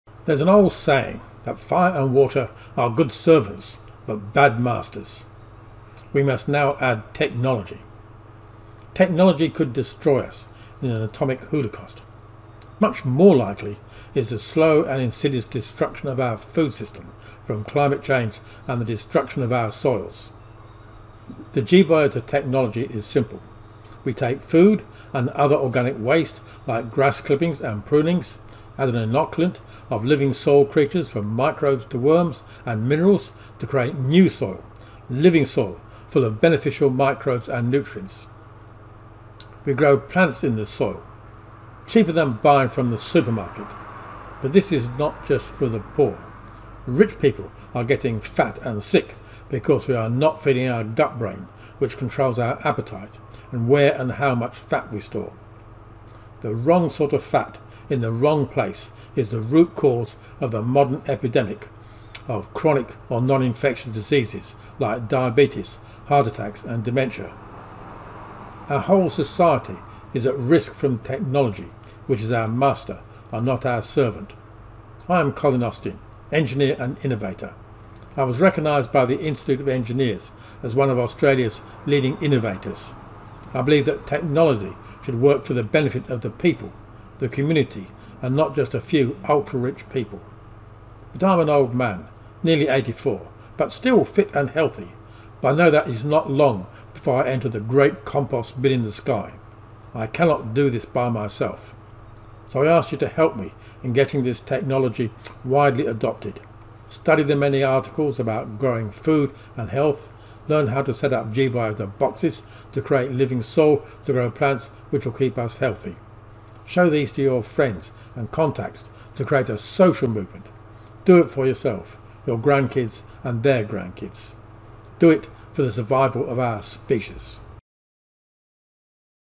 Fire, flood and technology
Technology should benefit the people.  Itis said that fire and flood are good servants but bad masters but we should add technology.  Here we show a technology that benefits the community